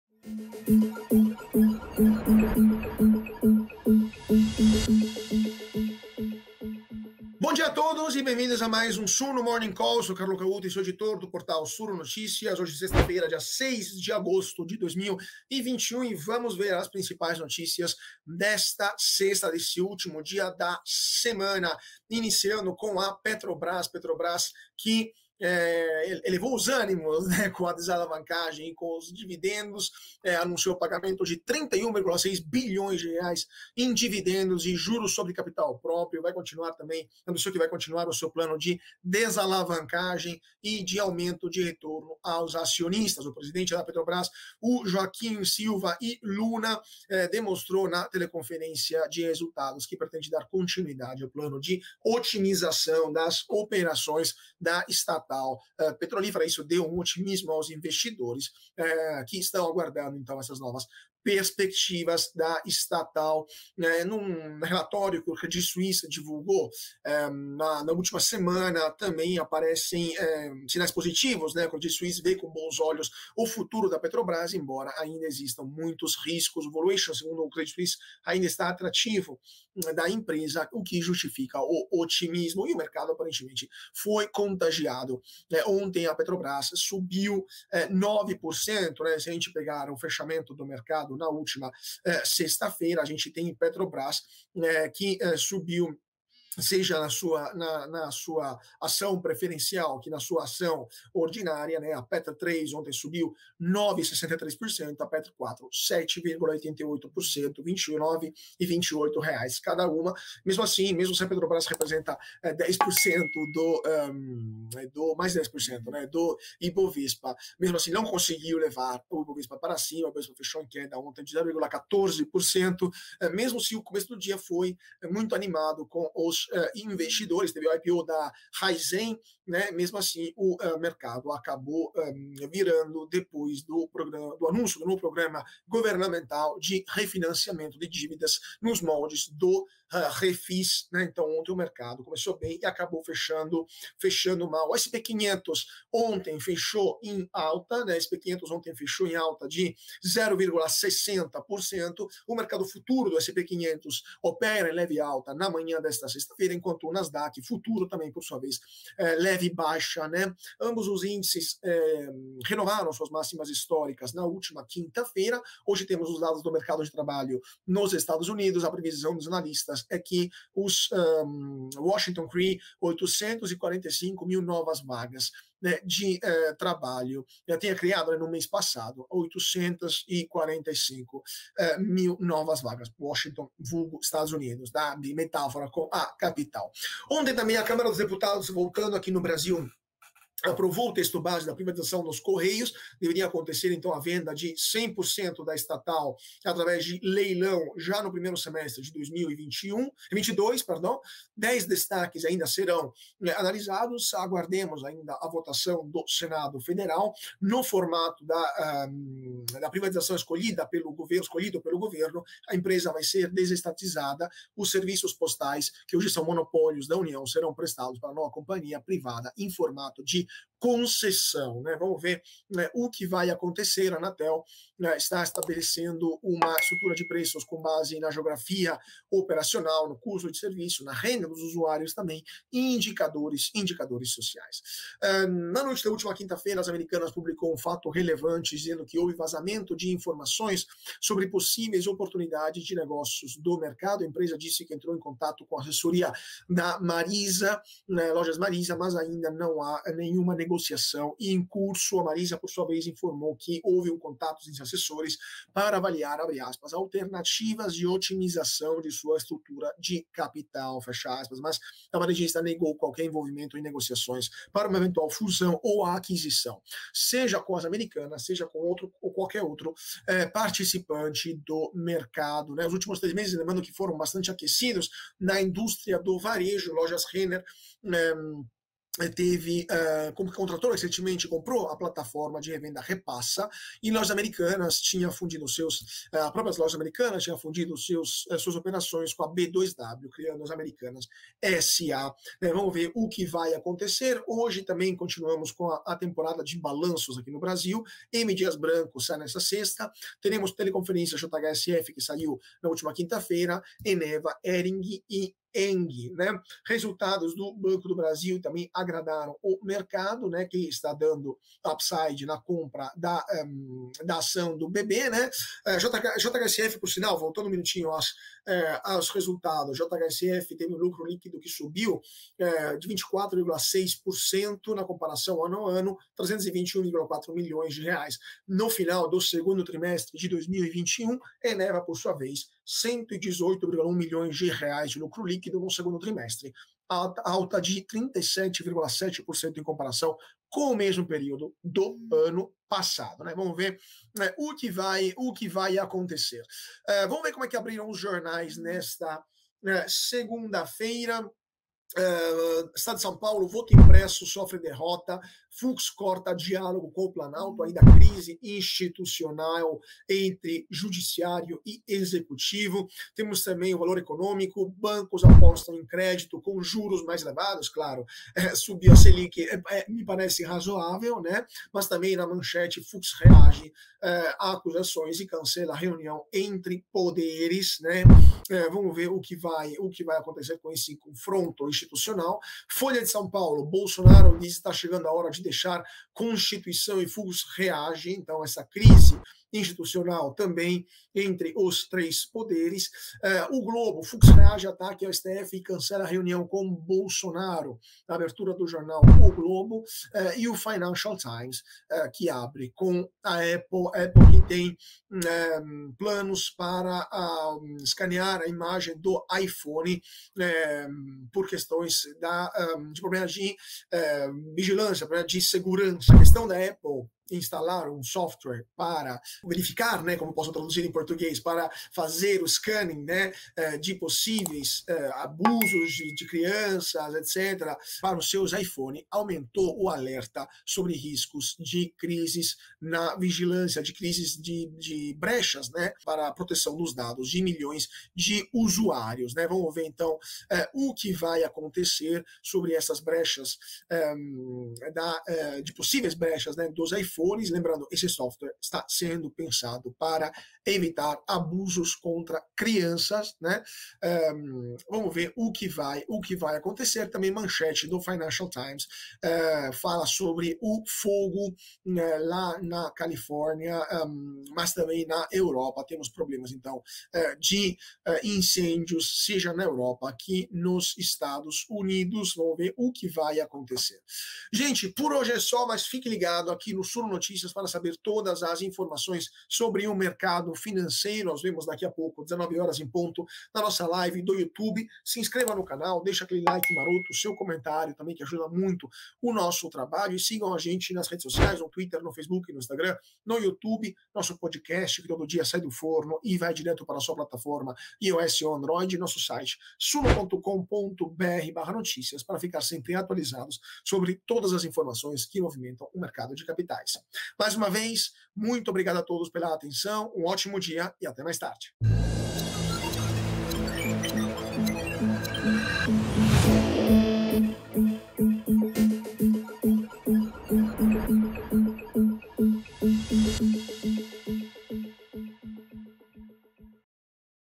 PETROBRAS ANUNCIA O PAGAMENTO DE R$ 31,6 BI EM DIVIDENDOS E JCP | PLANO DE DESALAVANCAGEM CONTINUA
📰 𝐒𝐔𝐍𝐎 𝐂𝐀𝐋𝐋 - 𝐓𝐮𝐝𝐨 𝐪𝐮𝐞 𝐯𝐨𝐜ê 𝐩𝐫𝐞𝐜𝐢𝐬𝐚 𝐬𝐚𝐛𝐞𝐫 𝐩𝐚𝐫𝐚 𝐜𝐨𝐦𝐞ç𝐚𝐫 𝐨 𝐝𝐢𝐚 𝐮𝐦 𝐩𝐚𝐬𝐬𝐨 à 𝐟𝐫𝐞𝐧𝐭𝐞 19 julho

SUNO CALL - NOTÍCIAS DE 06 DE AGOSTO, SEXTA-FEIRA, 2021

Mais uma semana chega ao fim. Entre altos e baixos, o Ibovespa é influenciado negativamente pelos bastidores de Brasília, que ameaçam a saúde fiscal no Brasil, mas dados corporativos fortes impedem uma queda mais forte do índice brasileiro.

Contraponto. Ontem, o Ibovespa caiu 0,14% após iniciar o dia com forte ânimo dos investidores. A virada de mão foi resultado do anúncio do novo programa governamental de refinanciamento de dívidas nos moldes do Refis. Por outro lado, a queda do índice só não foi mais feia em função da Petrobras (PETR4), que subiu quase 10%. Veja os destaques do fechamento do mercado.

Atividade. O mercado futuro do S&P 500 opera em leve alta na manhã desta sexta-feira, enquanto a Nasdaq, por sua vez, apresenta uma pequena baixa. Ambos os índices renovaram suas máximas históricas na véspera. Hoje, as atenções ficam voltadas aos dados do mercado de trabalho. Estima-se que os Estados Unidos criaram 845 mil novas vagas no mês passado.

00:00 - Introdução
00:21 - Petrobras eleva ânimos com desalavancagem e dividendos
02:24 - S&P 500 opera em leve alta na manhã desta sexta-feira
03:06 - Câmara aprova texto-base e abre caminho para privatização dos Correios
03:55 -  Americanas (AMER3) publicou um fato relevante dizendo que houve "vazamento de informações" sobre possíveis oportunidades de negócio no mercado
04:53 - A temporada de balanços continua a todo vapor
05:46 - O QUE ACONTECE HOJE
07:53 - Conclusão